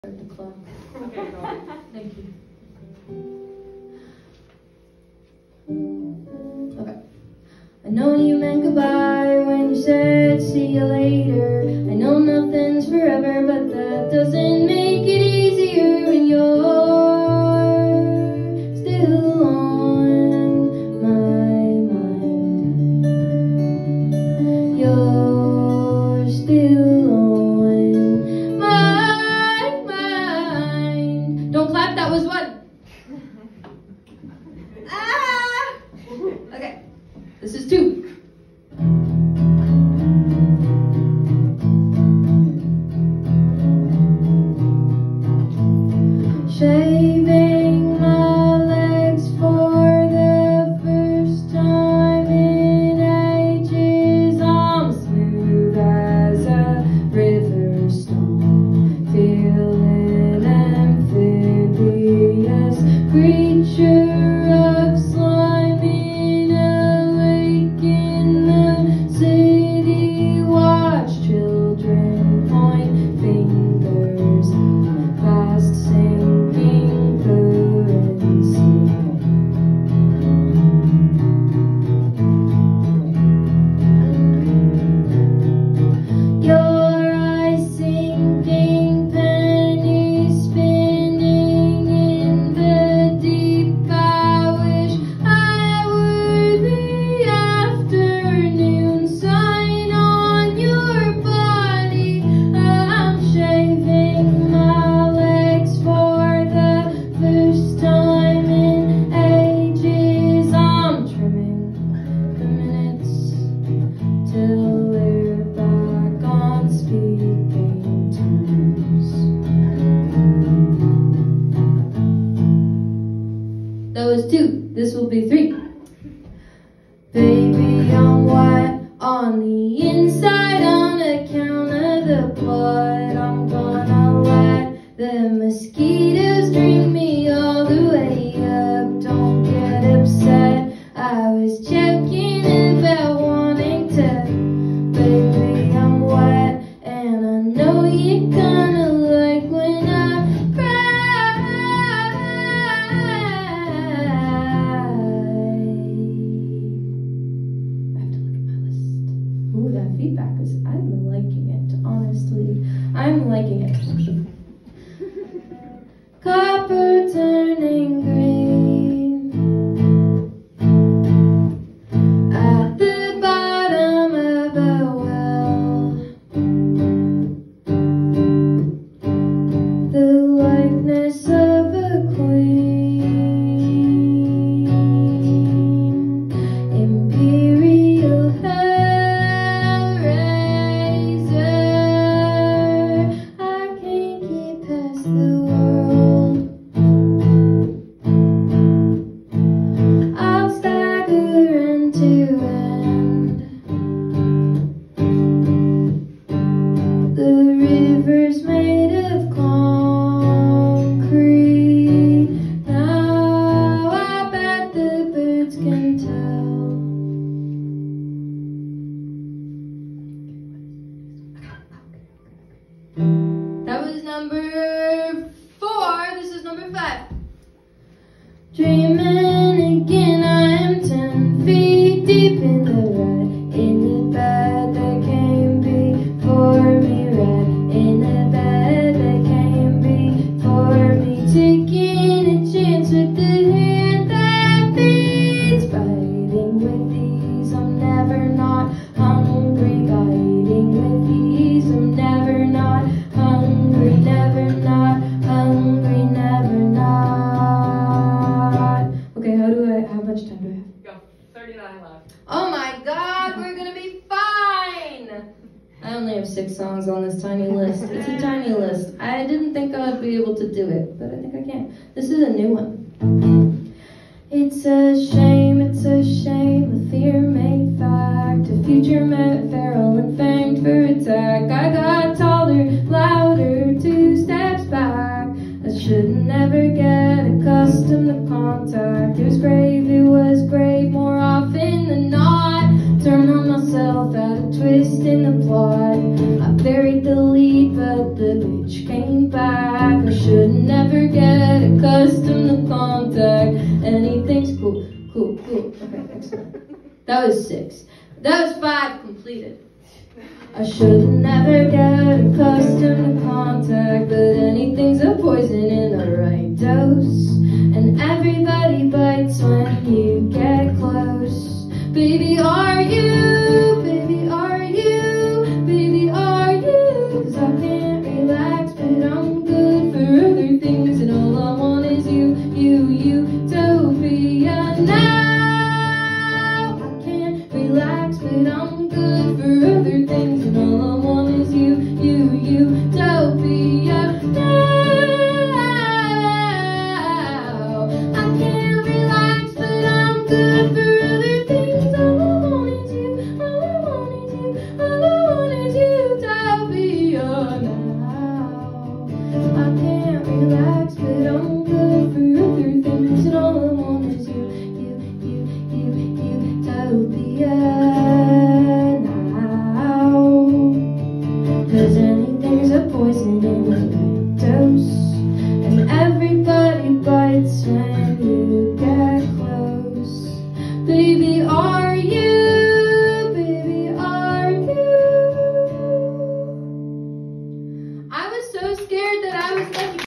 Rosie Tucker - Six Tiny Songs
Rosie Tucker performing six separate tiny songs in 6 min 30 sec. I’ll update the names if I get them.

Venue: The Mr. Roboto Project
Organized by: DLTSGDOM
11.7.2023